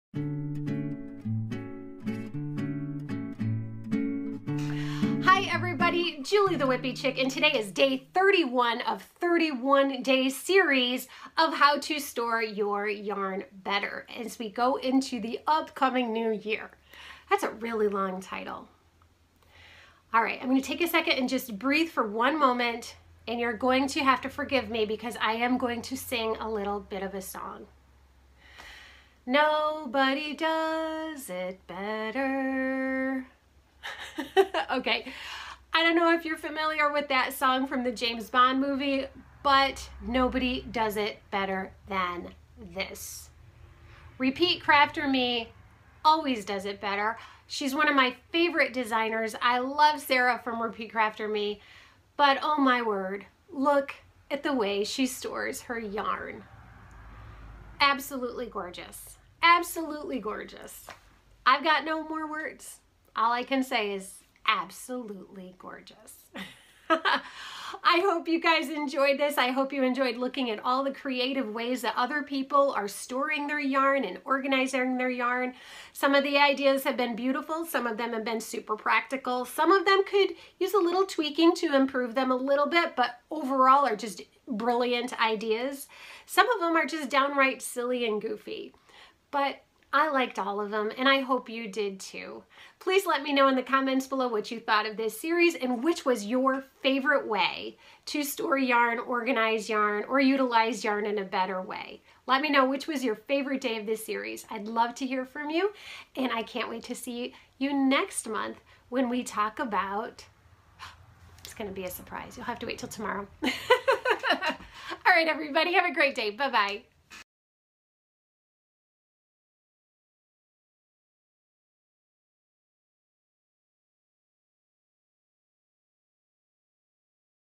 How to Organize Your Yarn Stash/#31 Nobody does it better than Repeat Crafter Me!
How to Organize Your Yarn Stash is a 31 day series that will give you a new creative idea to organize your stash every day! I hope you enjoy all the ideas in this series. I loved falling down this "rabbit hole" and looking at all the fun ideas!